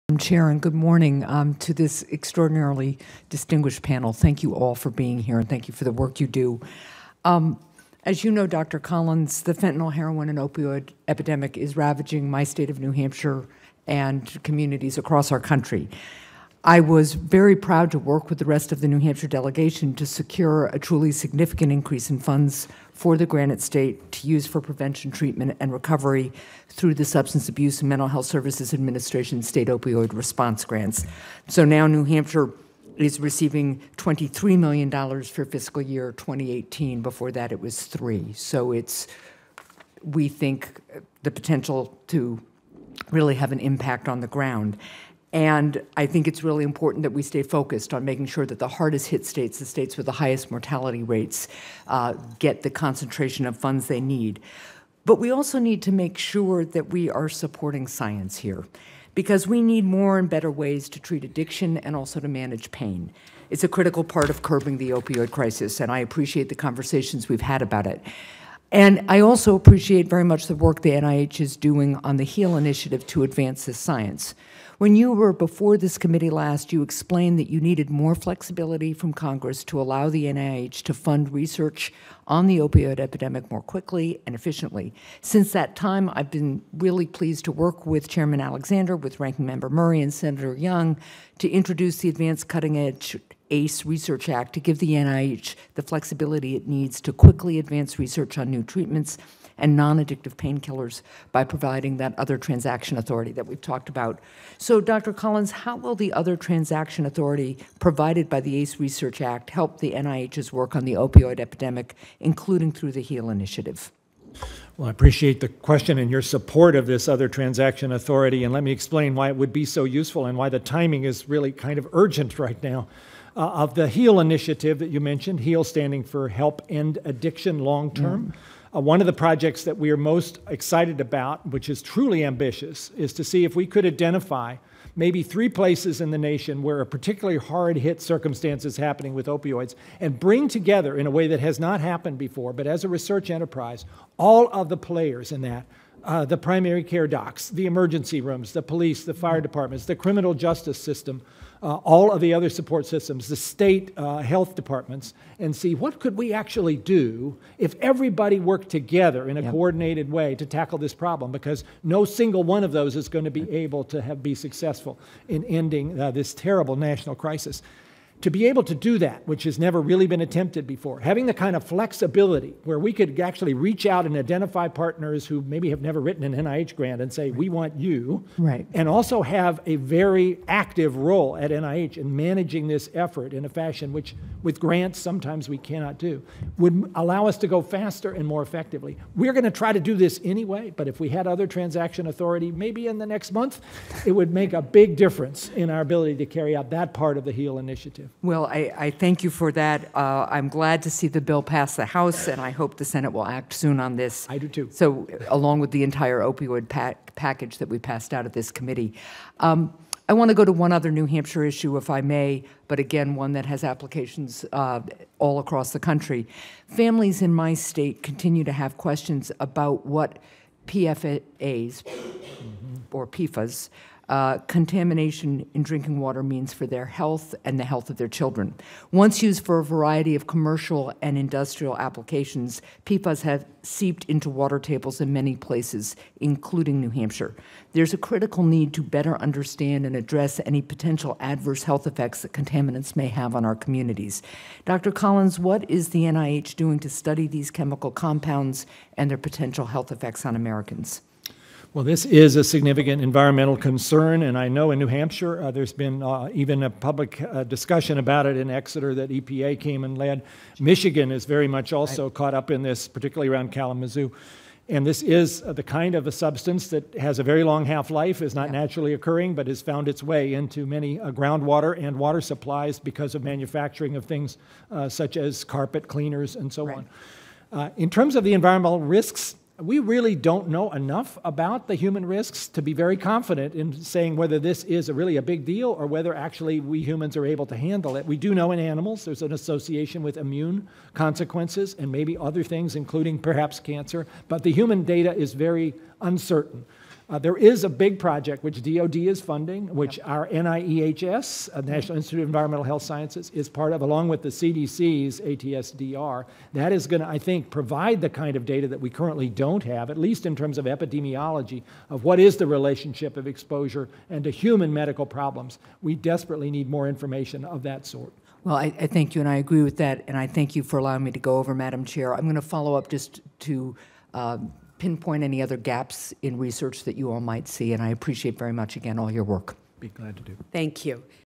Sen Hassan Pushes Director of NIH on Steps Agency is Taking to Better Understand Impacts of PFAS
Senator Maggie Hassan participated in a Senate Health, Education, Labor, and Pensions Committee hearing, where she pushed Dr. Francis Collins, Director of the National Institutes of Health (NIH), about the steps the agency is taking to understand how per and polyfluoroalkyl substances (PFAS) contamination in drinking water is impacting the health of Granite Staters and Americans.